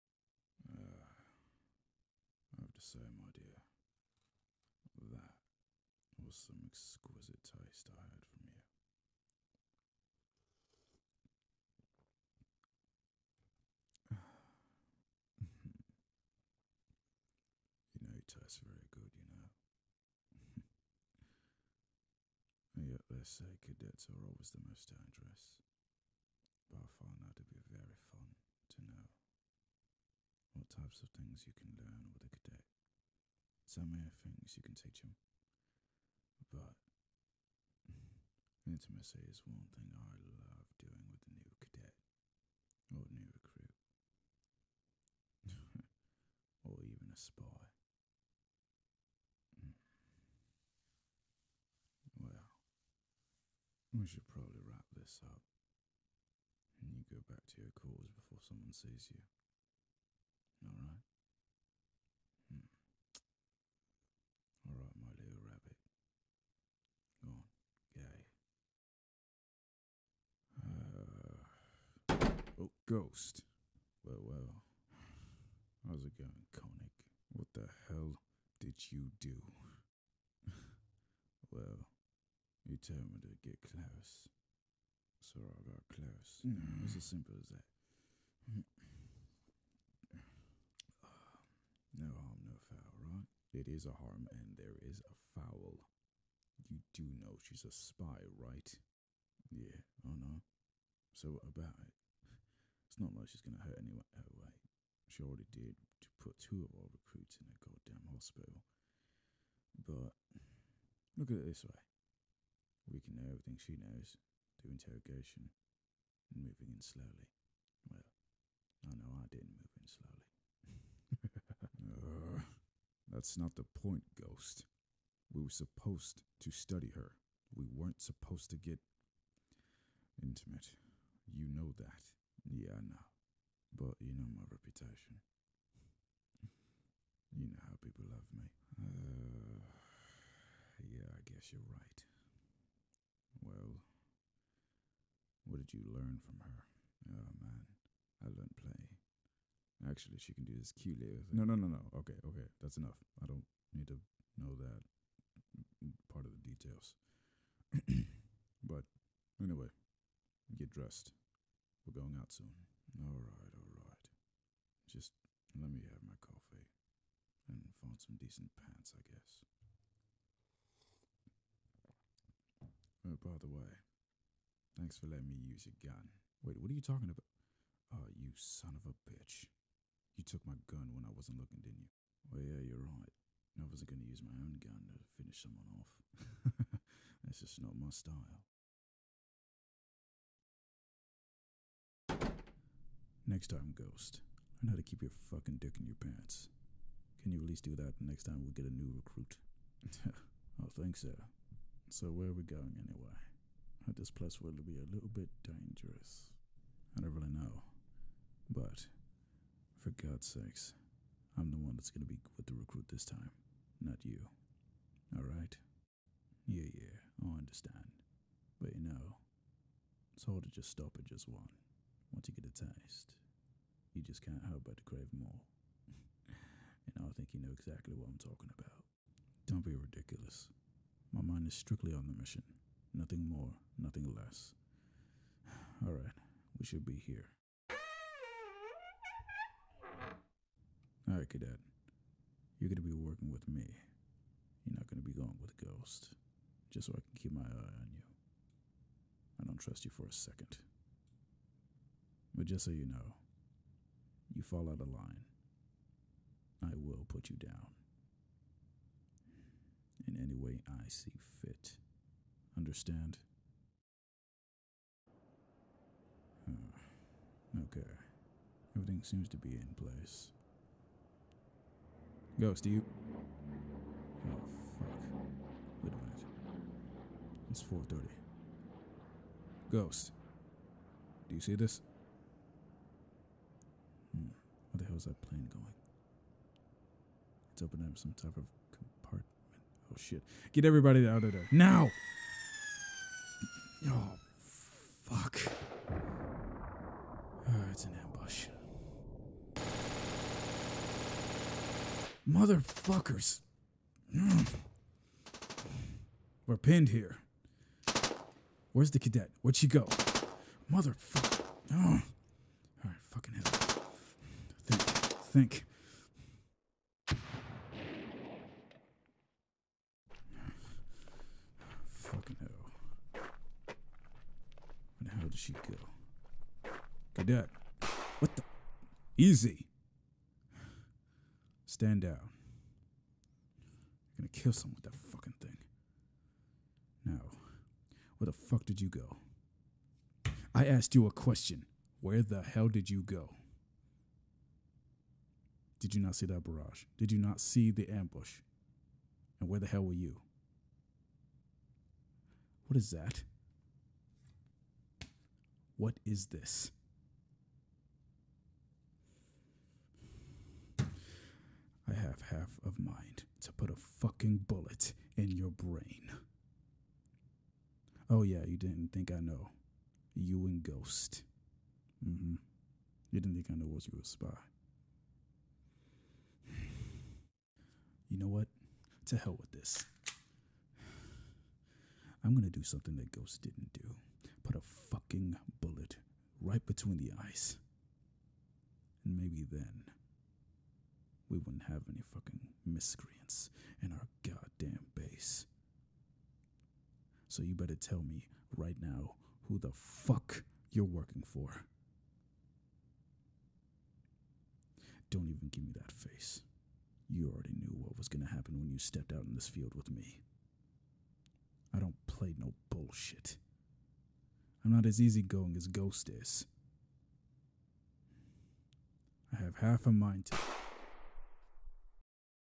Konig & Ghost ep 4 5 ASMR MP4
After Ghost has a passionate altercation with His "little Rabbit" Konig has decided to take the new recruit out in the field but finds themselves in an ambush, who knows what could happen in this episode.